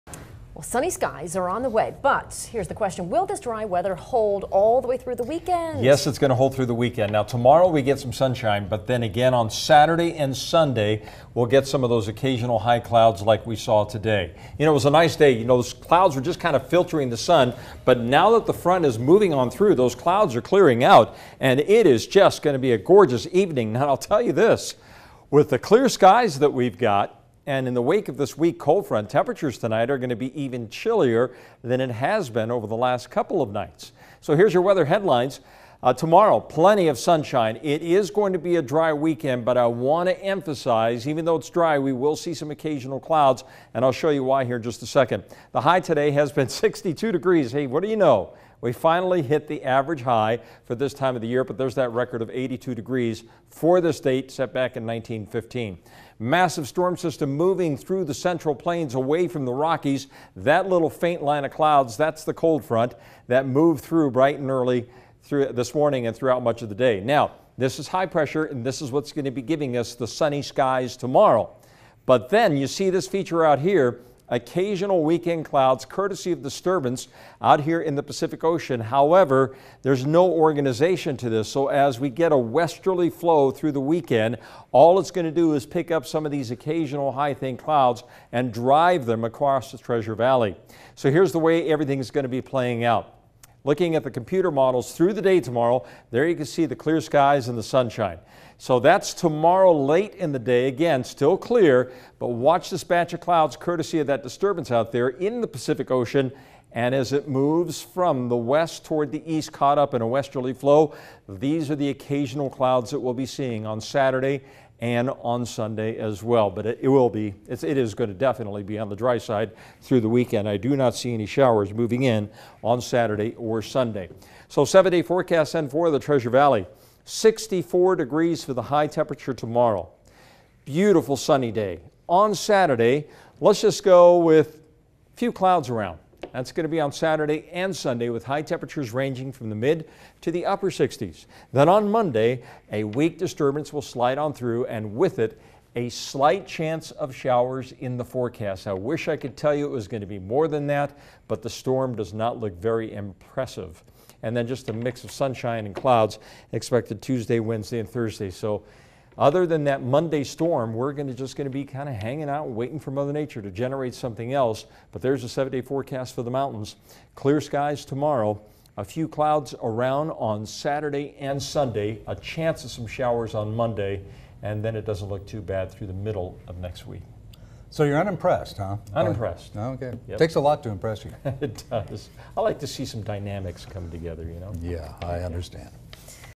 Idaho Evening Weather: October 22
Sunny and cool tomorrow.